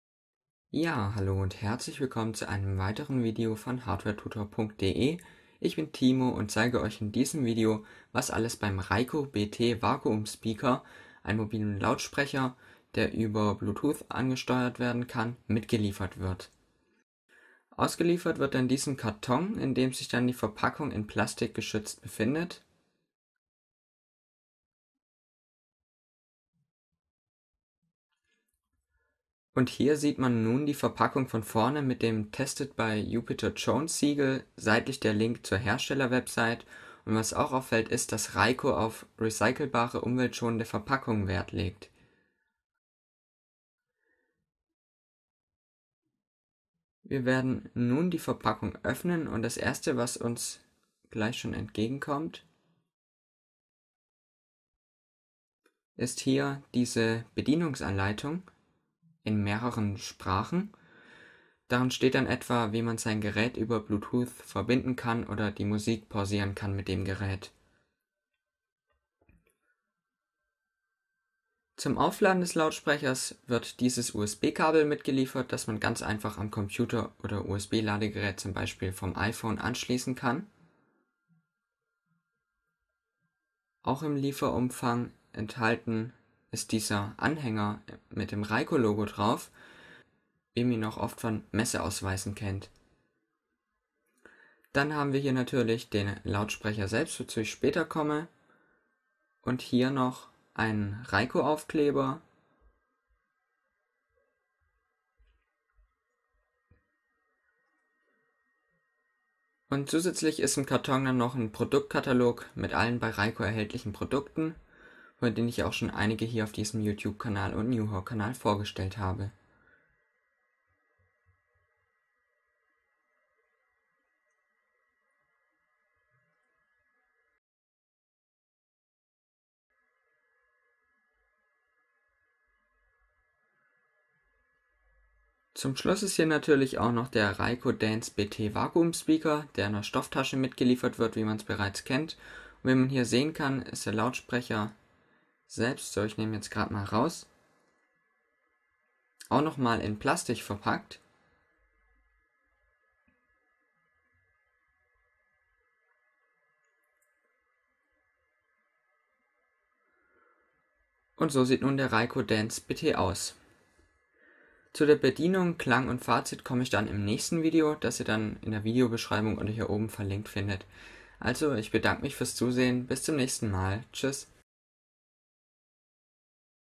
Raikko Dance Bluetooth Vacuum Speaker Unboxing
In diesem Video zeige ich euch den Lieferumfang des Raikko Dance BT Vakuum Speakers, der Musik via BLuetooth vom Smartphone, etc. abspielt.